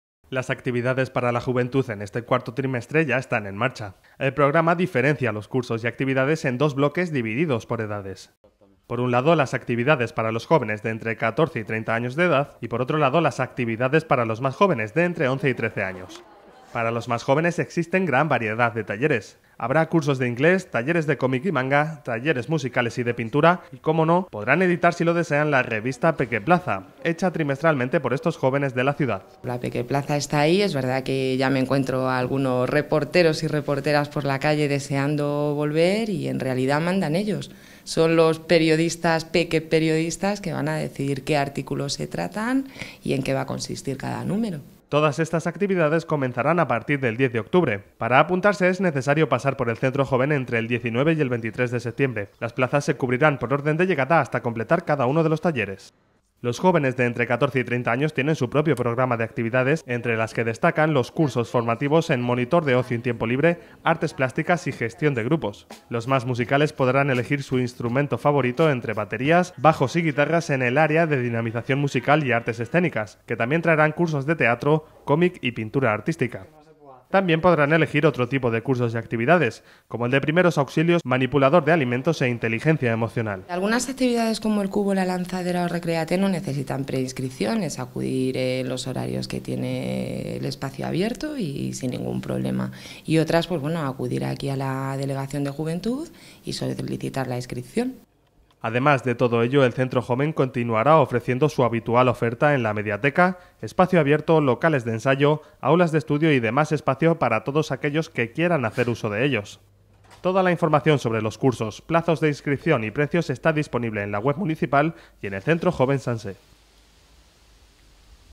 Ya está disponible el programa de actividades juveniles de otoño
David CAZALLAS / San Sebastián de los Reyes, Madrid / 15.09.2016
Las actividades para la juventud en este cuarto trimestre ya están en marcha. El programa diferencia los cursos y actividades en dos bloques divididos por edades. Por un lado, para los jóvenes de entre 14 y 30 años de edad, y por otro lado las actividades para chicos y chicas de entre 11 y 13 años.

Para los más jóvenes existen gran variedad de talleres. Habrá cursos de inglés, talleres de comic y manga, talleres musicales y de pintura y, como no, podrán editar si lo desean la revista Pequeplaza. Utilizarán también espacios gratuitos, como El Cubo y La Lanzadera para la realización de trabajos y dinámicas de ocio en el nuevo Centro Cívico en Dehesa Vieja.Todas estas actividades comenzarán a partir del 10 de octubre. Para apuntarse, es necesario apuntarse en el Centro Joven entre el 19 y 23 de septiembre. Las plazas se cubrirán por orden de llegada hasta completar cada uno de los talleres.

Los jóvenes de entre 14 y 30 años tienen su propio programa de actividades, entre las que destacan los cursos formativos en monitor de ocio y tiempo libre, artes plásticas y gestión de grupos. Los más musicales podrán elegir su instrumento favorito entre baterías, bajos y guitarras  en el área de dinamización musical y artes escénicas, que también traerán cursos de teatro, cómic y pintura artística, entre otras muchas actividades.

El Centro Joven, además, continuará ofreciendo su habitual oferta en la Mediateca, Espacio Abierto, Locales de Ensayo, Aulas de Estudio y demás espacios para todos aquellos que quieran hacer uso de ellos. Toda la información sobre los cursos, plazos de inscripción y precios está disponible en la web municipal y en el Centro Joven Sanse.

Centro Joven Sanse
Avda. Valencia, 3